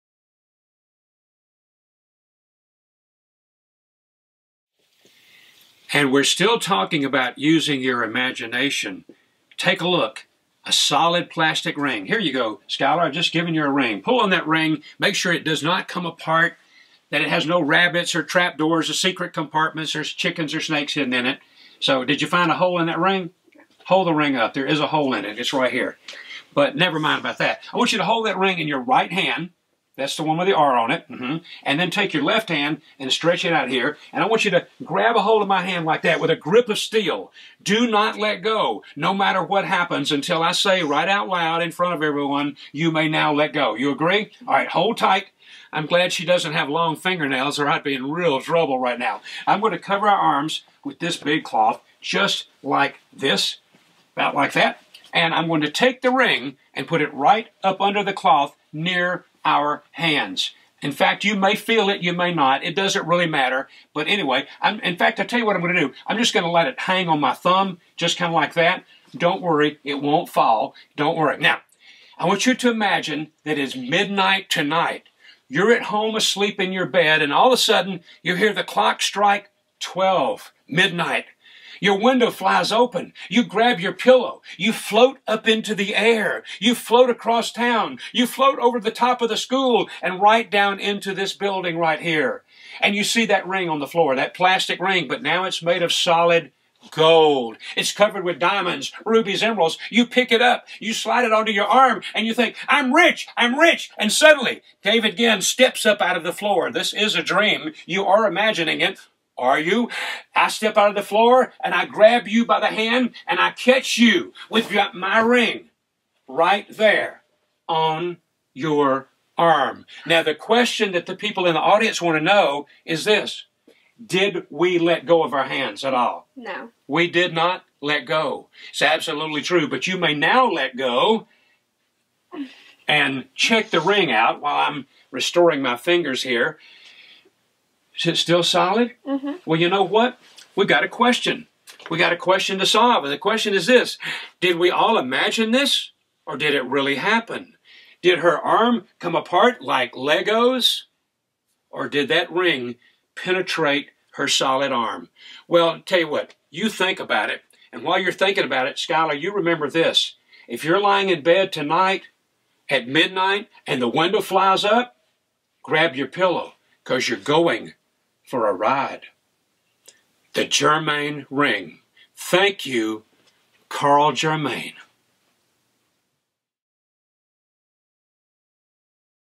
David Ginn Magic #181 Germain Ring
David Ginn Magic #181:  Germain Ring.  Having read Stuart Kramer’s book,  The Secrets of Karl Germain, as a college student, I developed the routine you see here from the book description. It worked well for me in over 1000 live shows. A full explanation will appear in Kidshow Magic Kompendium, to be published the summer of 2017.